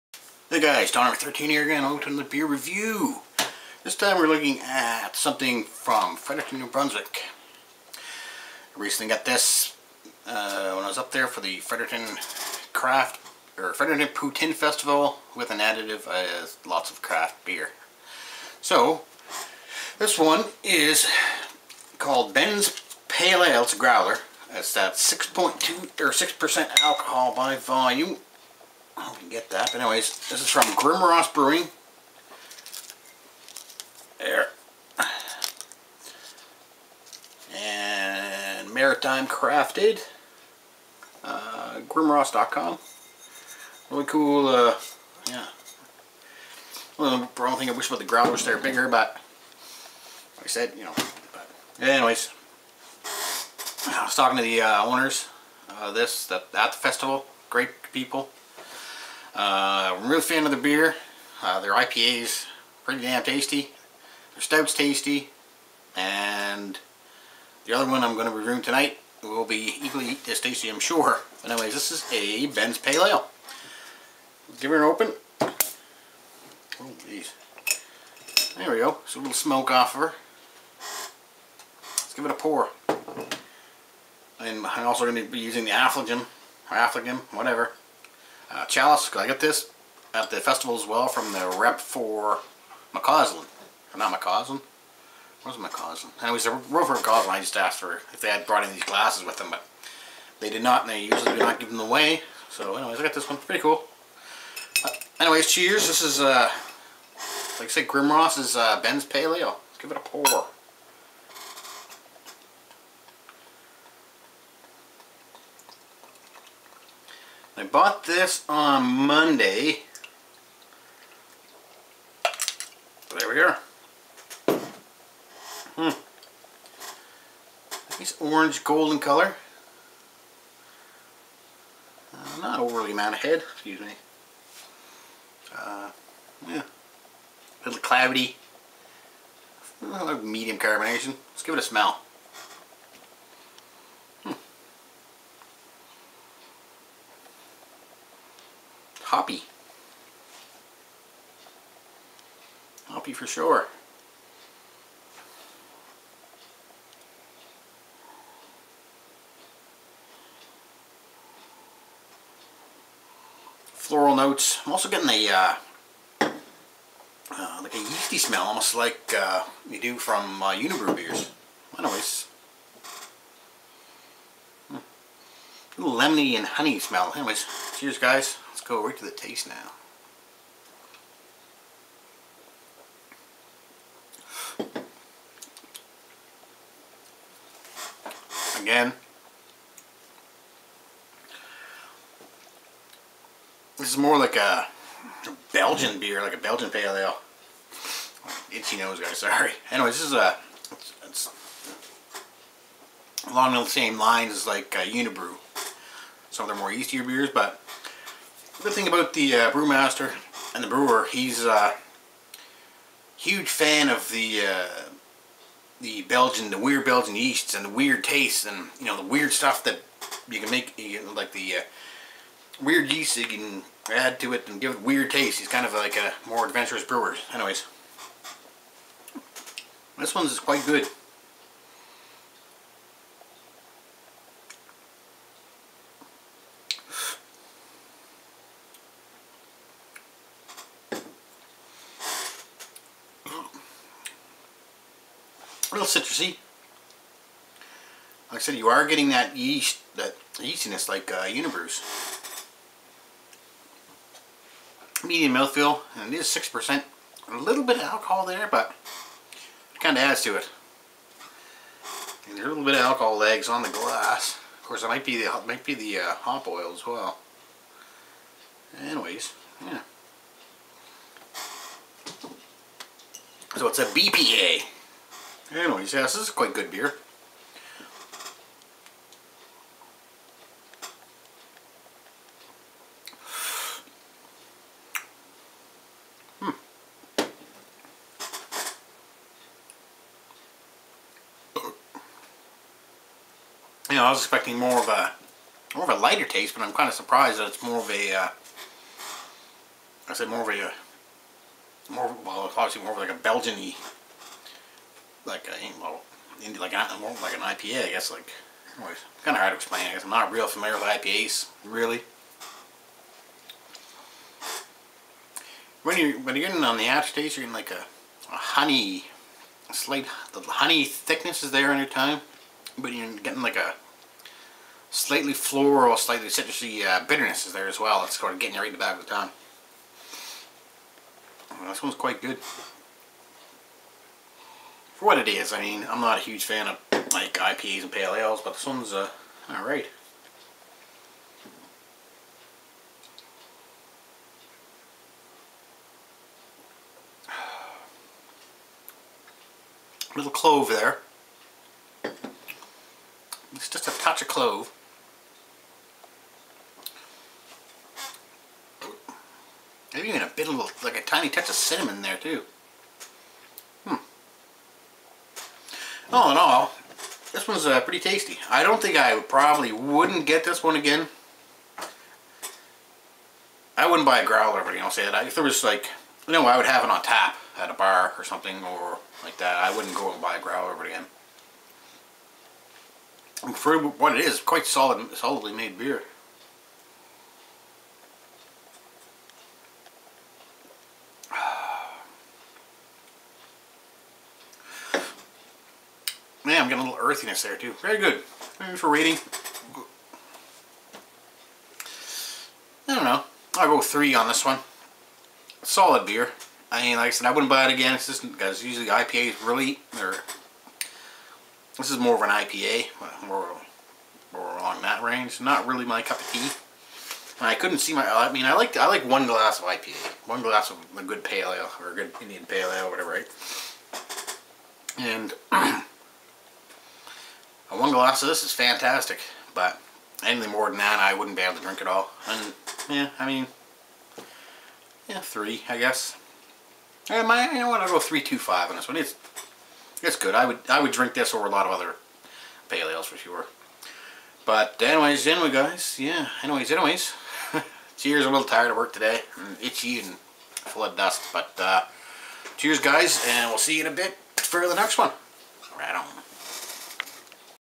Grimross' Bens Pale Ale - Beer Review #233
Taking a look at an ale from Grimross Brewing out of Fredericton, New Brunswick!